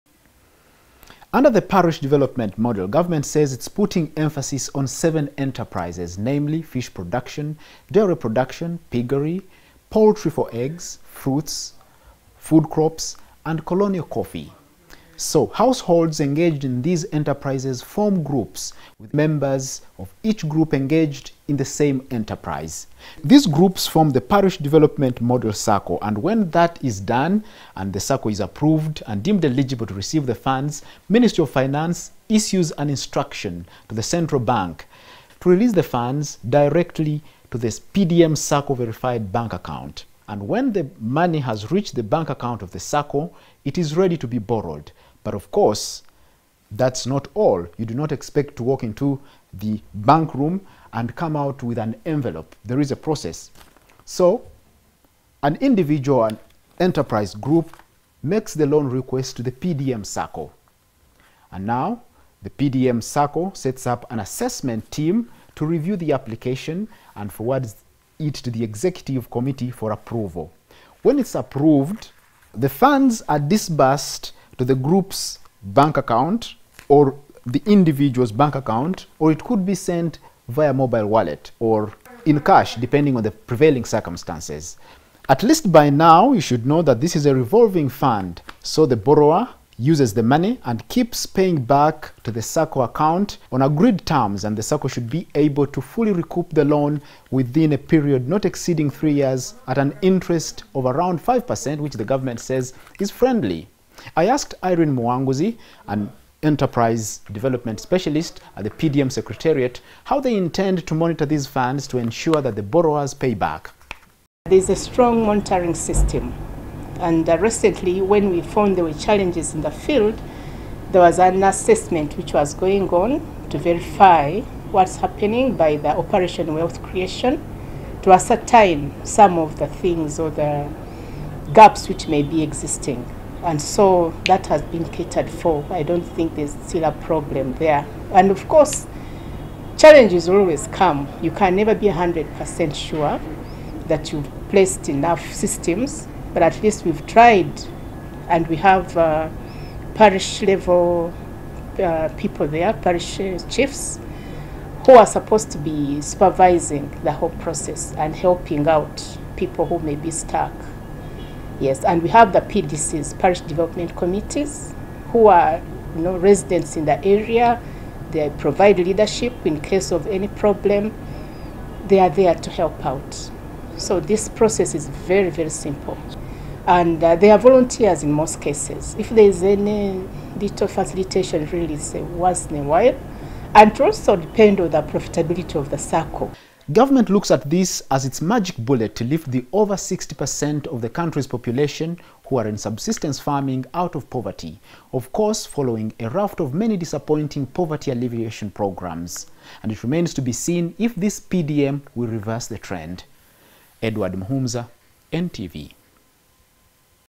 We track the PDM money and how it is accessed from secretariat
Over 70 per cent of the ten thousand-five hundred-and-ninety-four parishes have registered SACCOS to benefit from the revolving fund under Parish Development Model.

Irene Muwanguzi, an Enterprise Specialist at the PDM secretariat has told NTV that parishes that take a long to be ready may receive a hundred million shillings as a lump sum for the financial year instead of the four instalments sent now.

But what is the process for accessing this money?